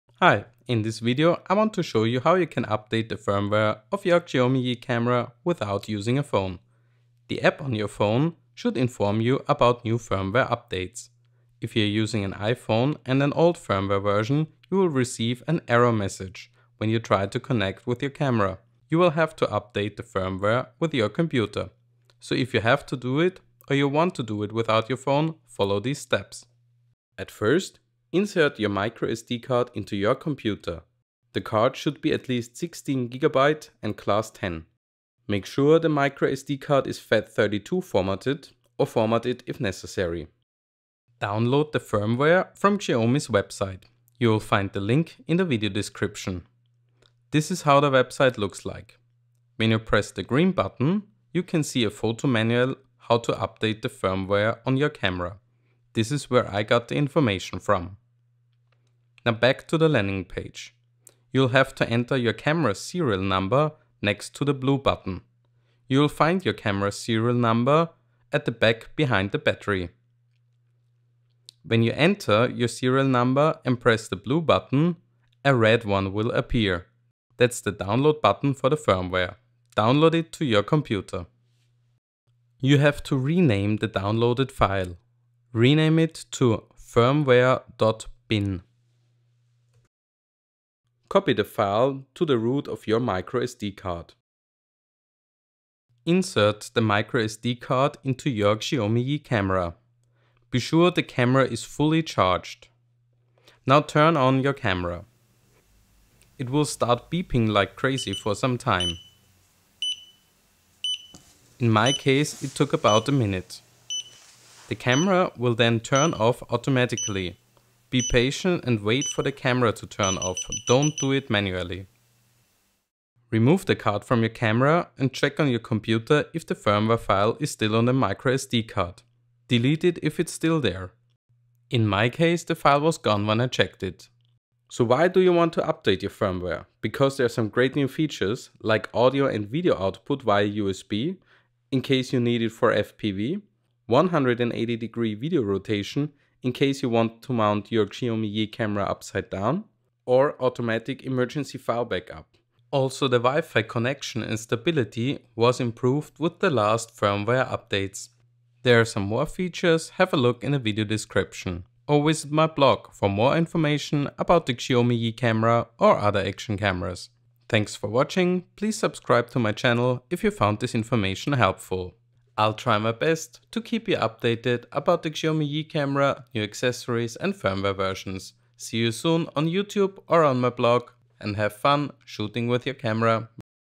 Xiaomi Yi Firmware Update - Tutorial
This is a tutorial video on how to update the firmware of your Xiaomi Yi camera with your computer.
The video was done with firmware 1.0.9, but the basics are the same.

Firmware Updates:
1.2.6
Fix freezing issue when booting camera
Fix firmware upgrade problem when terminating app while uploading
Fix ratio problem (4:3 instead of 16:9) of burst shot

1.2.0
video time-lapse
16:9 photos
2k video resolution

1.0.12
power LED flashes during video recording, when taking photos, before a self-timed photo
improved wifi transfer speed 6 stability
SD card problems fixed

1.0.9
Image/video rotation function
Automatic emergency file backup
Adjusted time stamp option
Fixed USB information error
Fixed charging light
Fixed Wi-Fi connectivity problem
Adjusted Wi-Fi function to achieve more stable connectivity
Improved mobile app and camera connectivity stability
Fixed mobile app file management instability
Fully English manual
A/V out via USB